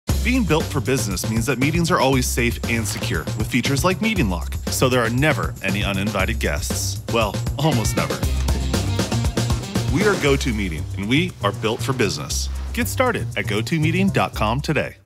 GoToMeeting is BuiltForBusiness | Work Securely, Anywhere
With GoToMeeting you can work from anywhere, securely. Get crystal clear audio and video and be assured that your meetings are safe and secure anywhere you choose to make work, work.